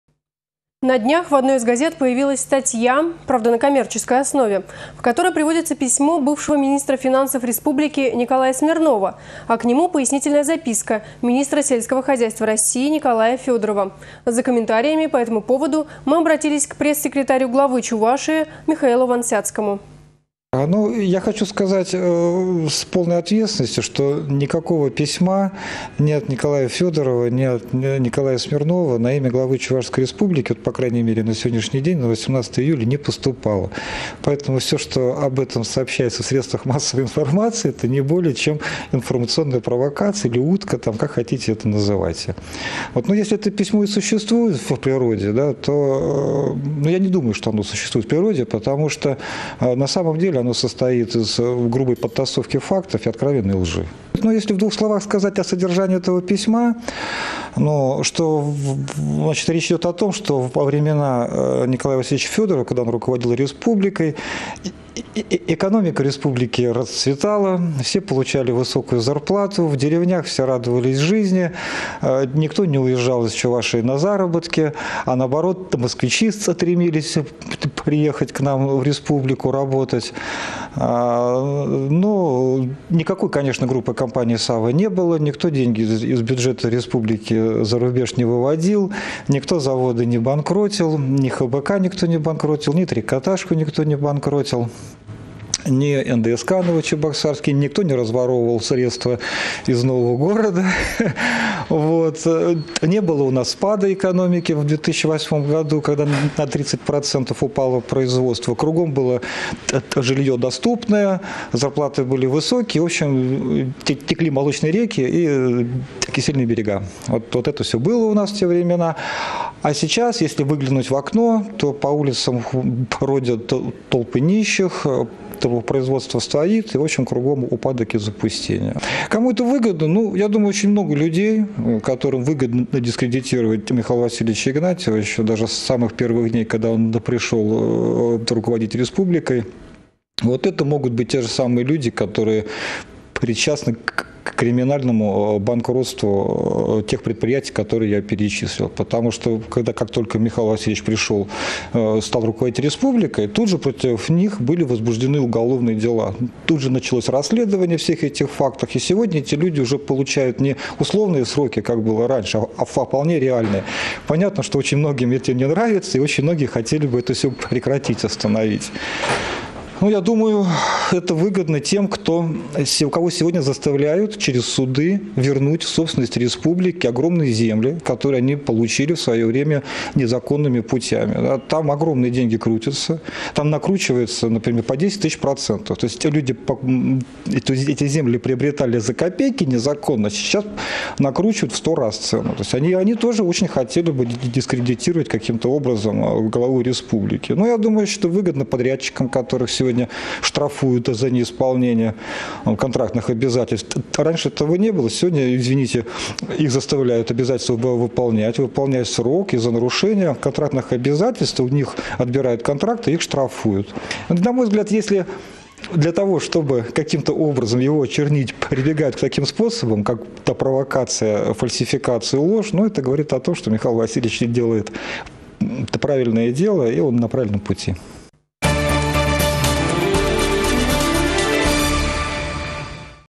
Михаил Вансяцкий о письме Н.Федорова М.Игнатьеву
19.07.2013
На днях в одной из газет появилась статья, правда на коммерческой основе, в которой приводится письмо бывшего министра финансов республики Николая Смирнова, а к нему - пояснительная записка министра сельского хозяйства России Николая Федорова. За комментариями по этому поводу мы обратились к пресс-секретарю Главы Чувашии Михаилу Вансяцкому.